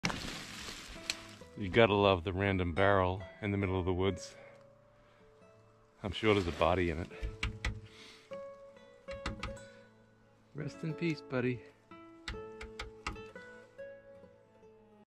Suspicious barrel!
I came across an old barrel in the middle of the forest